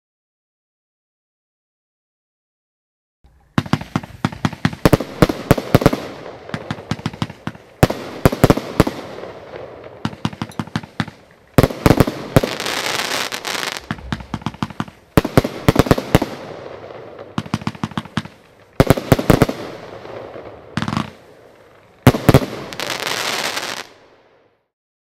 01250 Ultrablitz - Zena Vuurwerk [OFFICIAL VIDEO]
Dikke Zena bak met 36 schoten die in salvo’s van 6 weggeramd worden. De gouden palmen die eindigen in zilveren blinkers zijn bizar goed!